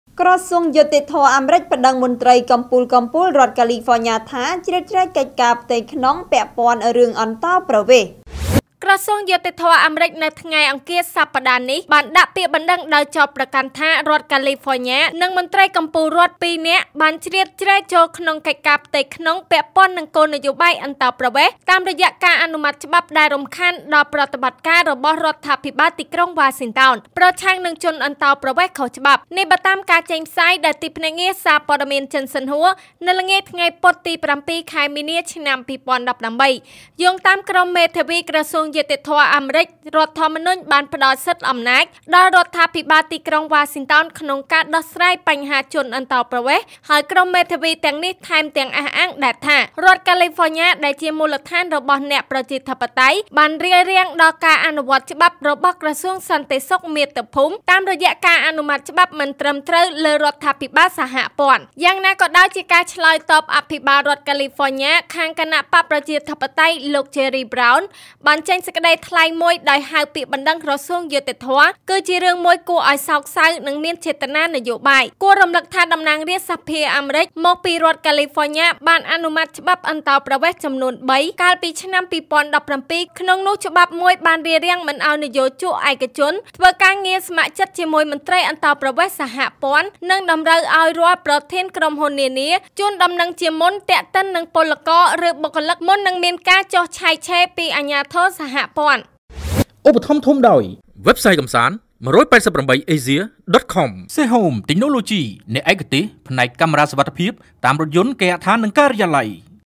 ក្រសួងយុត្តិធម៌អាមេរិក «ប្តឹងមន្ត្រីកំពូលៗរដ្ឋកាលីហ្វរញ៉ាថា ជ្រៀតជ្រែក​កិច្ចការផ្ទៃក្នុង...
ក្រសួងយុត្តិធម៌អាមេរិក «ប្តឹងមន្ត្រីកំពូលៗរដ្ឋកាលីហ្វរញ៉ាថា ជ្រៀតជ្រែក​កិច្ចការផ្ទៃក្នុង ពាក់ព័ន្ធ​រឿង​អន្តោប្រវែសន៍...» (រាយការណ៍ព័ត៌មានដោយកញ្ញា សំ ស្រីនីត​ និងអ្នកនាង កេត សត្យា, Fresh News TV)